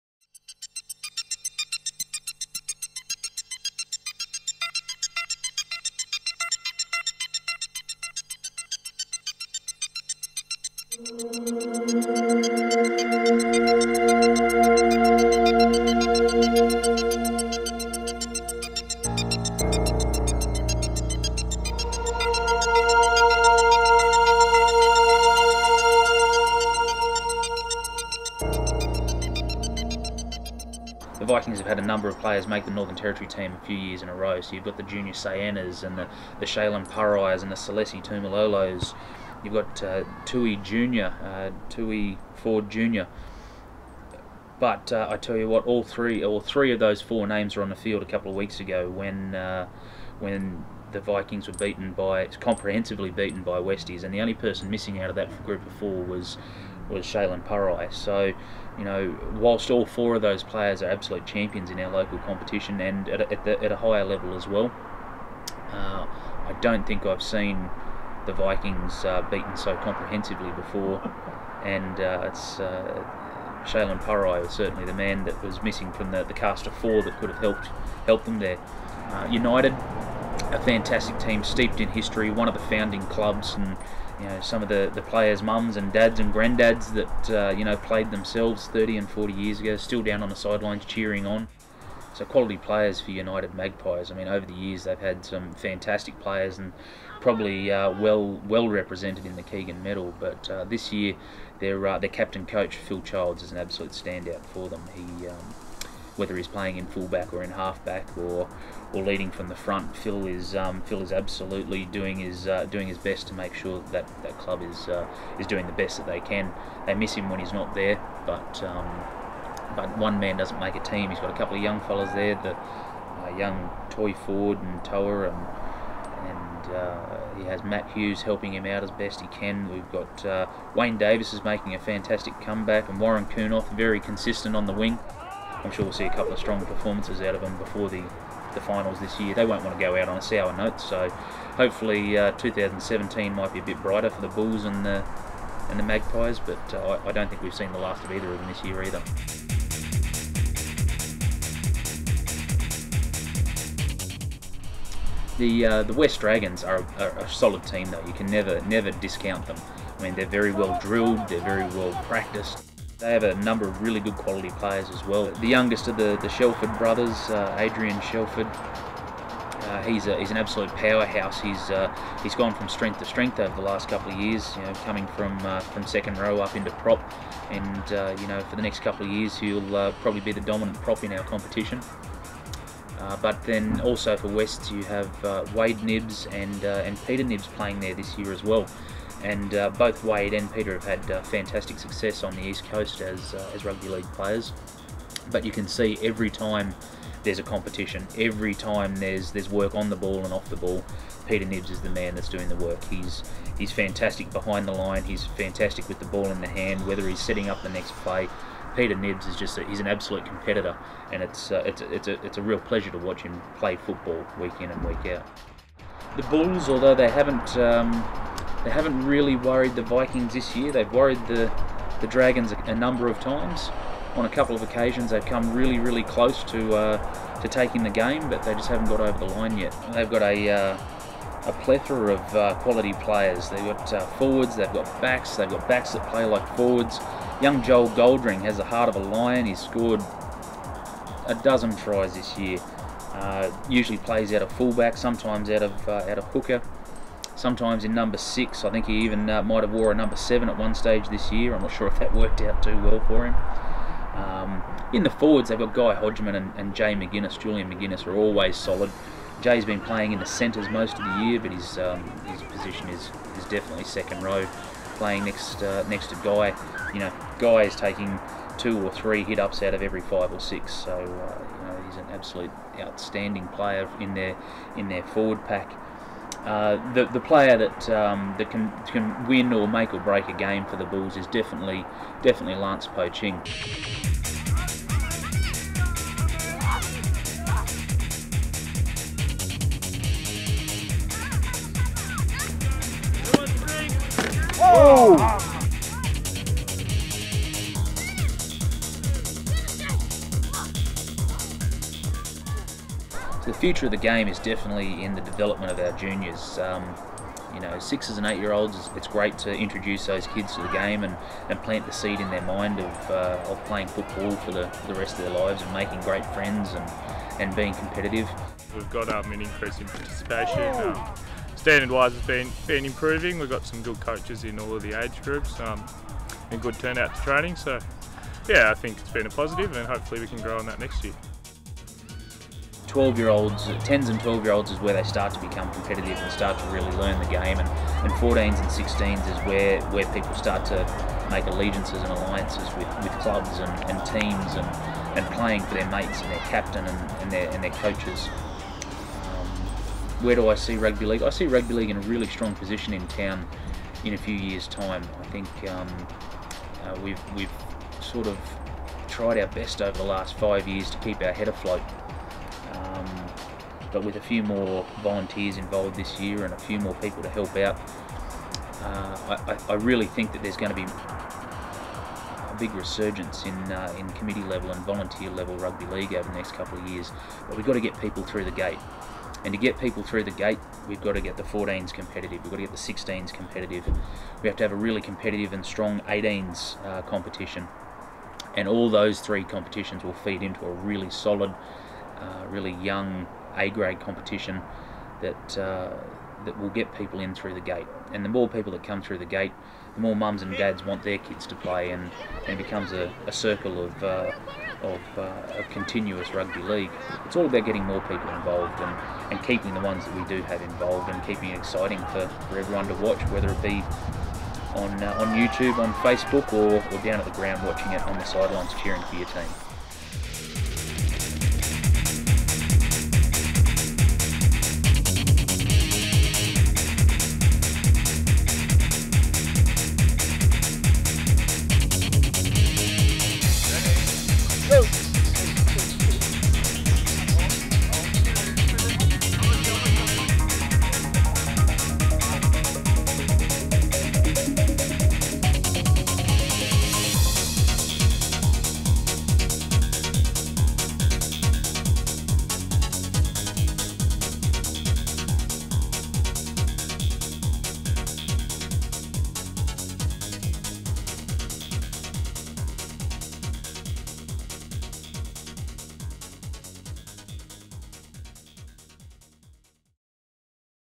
CARFL 2016 PROMO
Central Australian Rugby Football League 2016 promoting players and teams in Alice Springs..